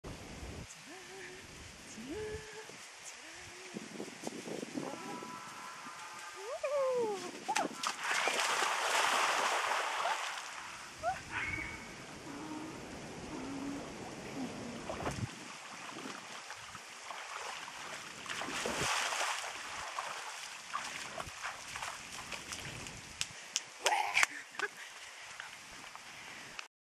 A pior estrela ever! Em Lençóis Maranhenses
A pior estrela do mundo... ever... kkk
Dando estrelinha e se empacotando no Igarapé de Atins - vilarejo dos Lençóis Maranhenses